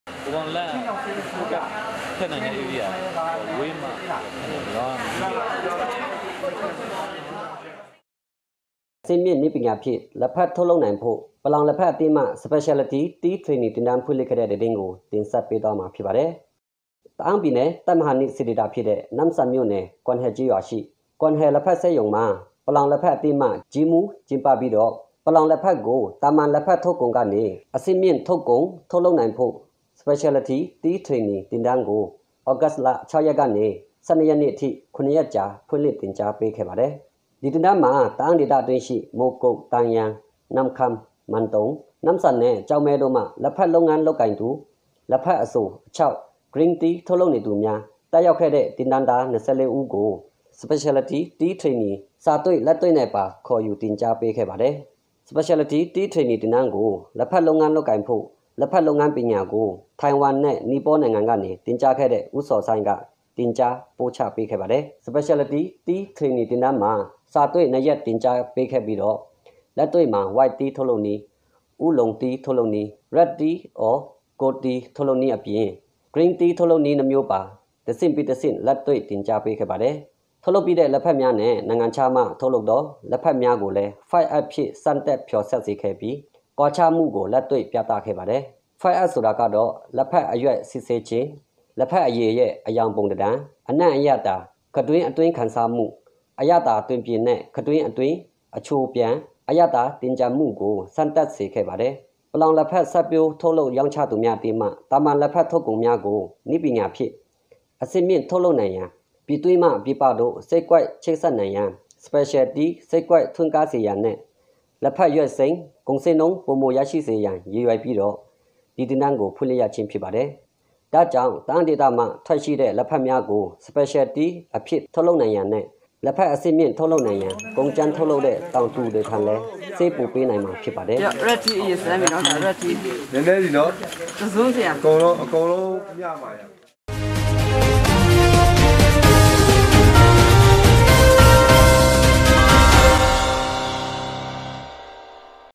အဆင့်မြင့်နည်းပညာဖြင့် ထုတ်လုပ်နိုင်ရန် ပလောင်လက်ဖက်အသင်းမှ Specialty Tea Training သင်တန်းဖွင့်လှစ်
Ta'ang State News ဗမာဘာသာ